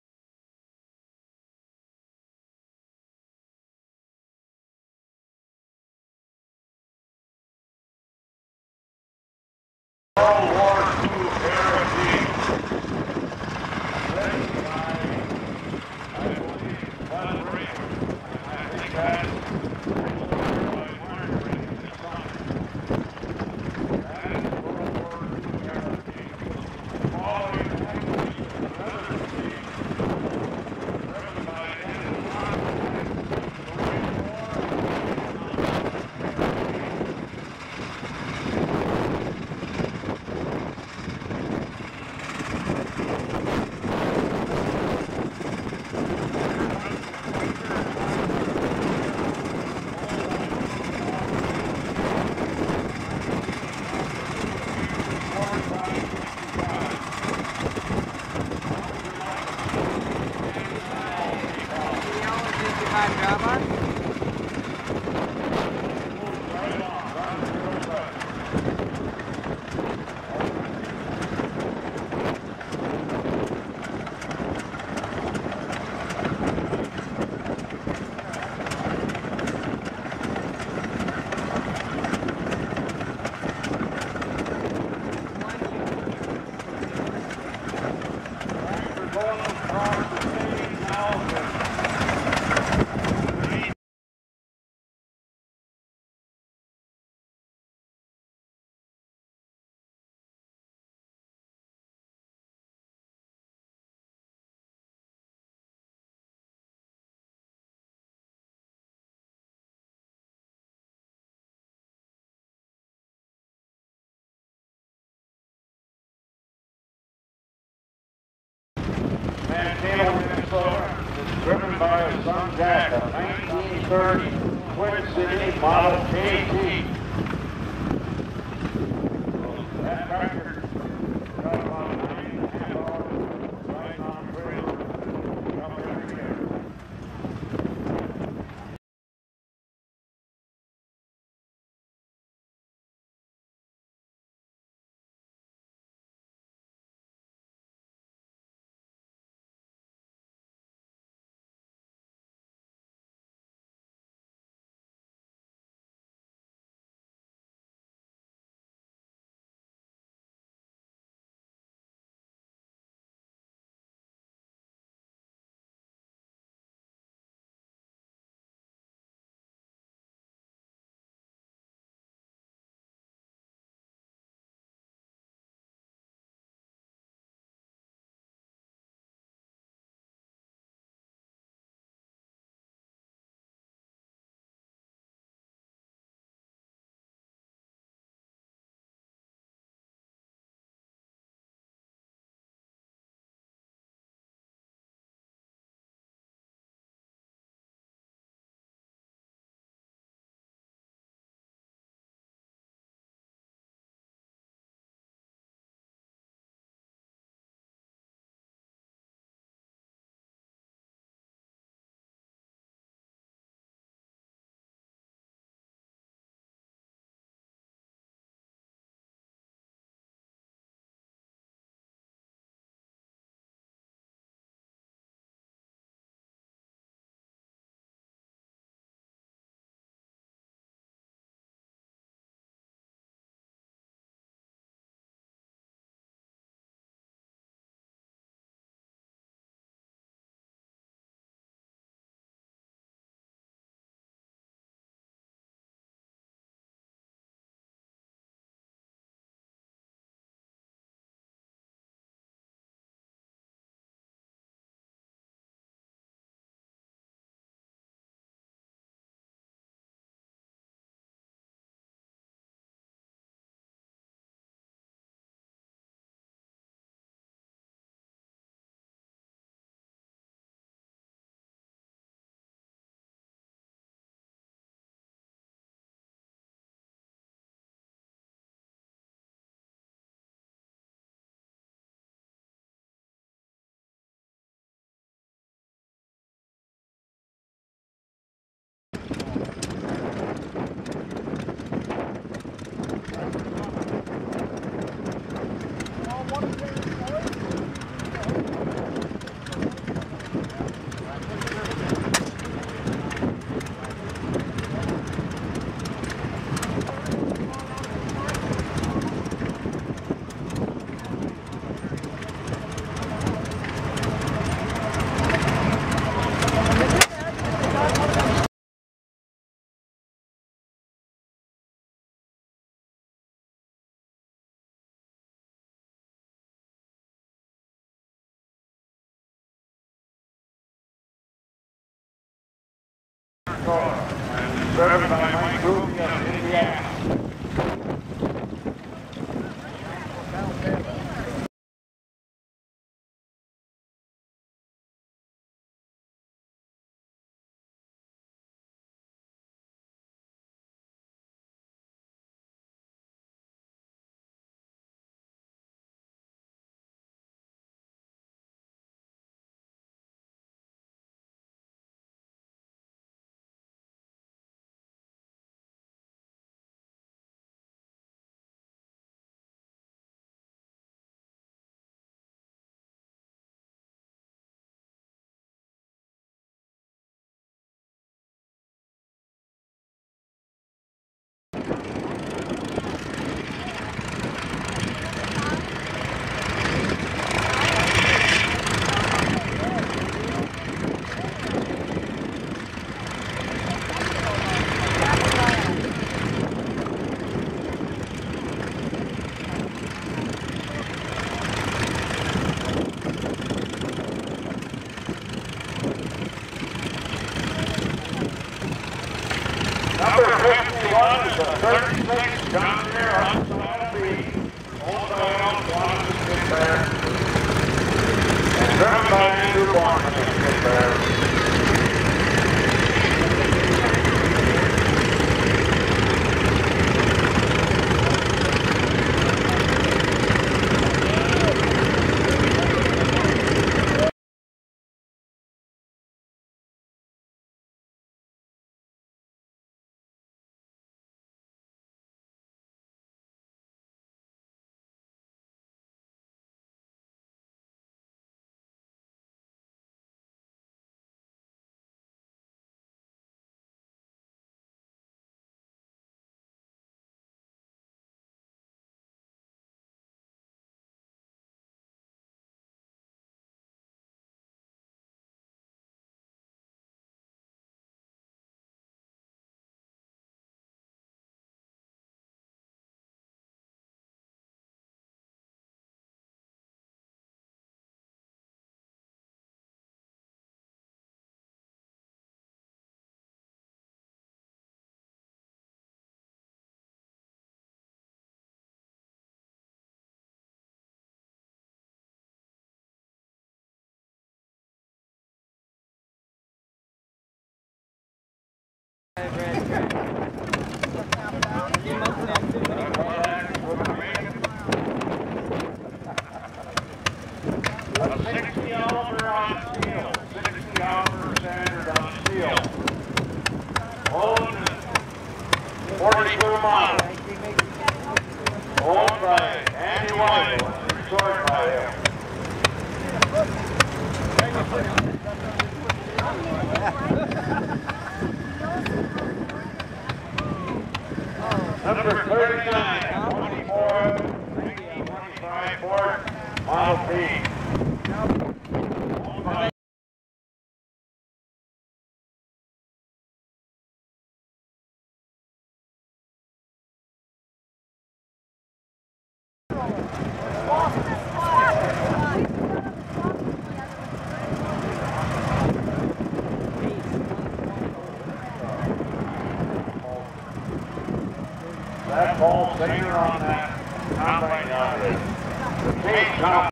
2010 Threshing Bee--The Parade!
Here is a compilation of photos and video clips from the 36th Annual South Central Threshing Bee in Braddock ND. This was taken Sunday, September 12, and shows the Antique Tractor Parade. Enjoy!